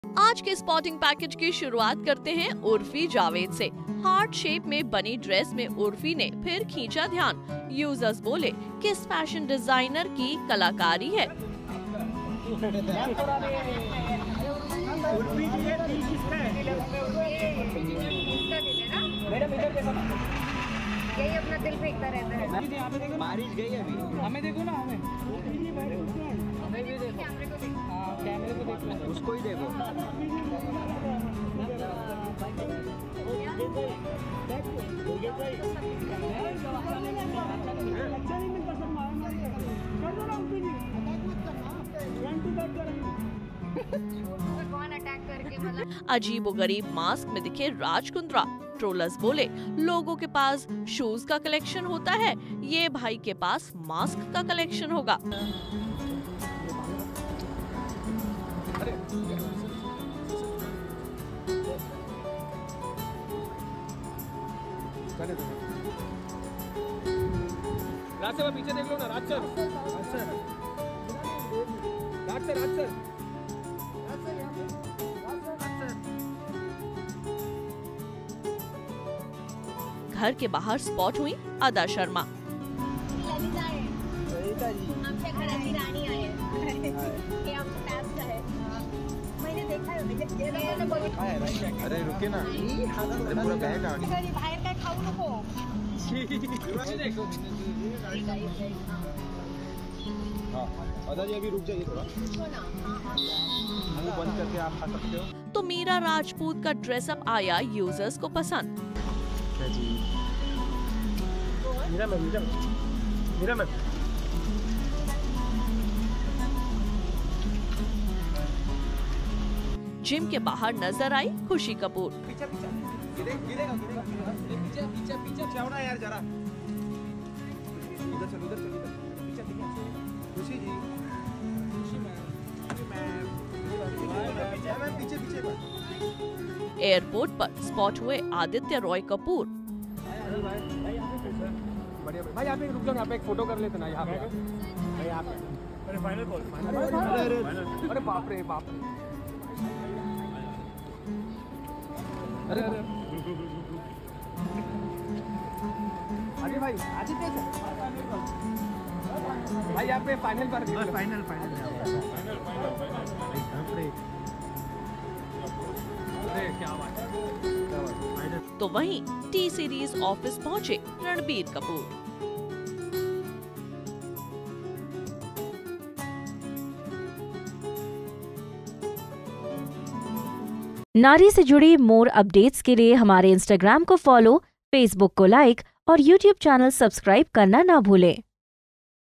किस फैशन डिजाइनर की कलाकारी है' दिल वाला outfit
.'किस फैशन डिजाइनर की कलाकारी है' दिल वाला outfit पहनकर सड़क पर निकली Uorfi Javed, मकड़ी वाले मास्क में चेहरा छुपाते दिखे Raj Kundra, 'The Kerala' फेम अदा शर्मा से पैपाराजी ने पूछा कुछ ऐसा शर्माते हुए बोलीं- ऐसे सवाल क्यों पूछते हो...